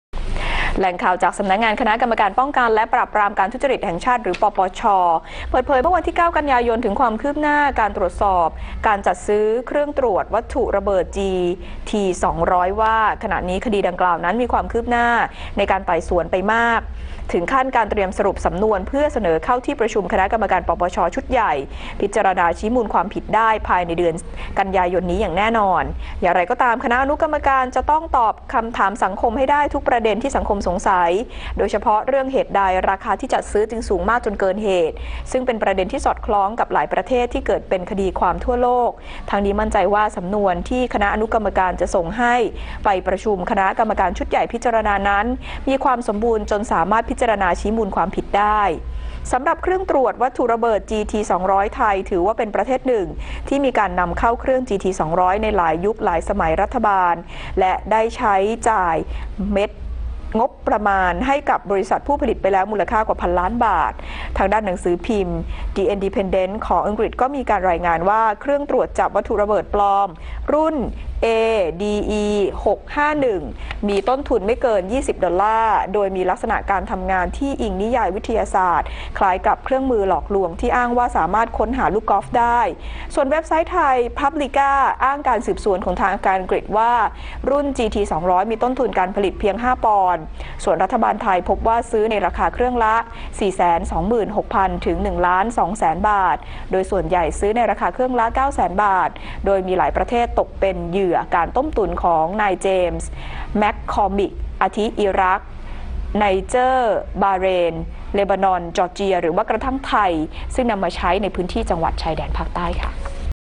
ป.ป.ช.เตรียมชี้มูลฯทุจริตจีที 200 ภายใน ก.ย.นี้
10/09/2016 ป.ป.ช.เตรียมชี้มูลฯทุจริตจีที 200 ภายใน ก.ย.นี้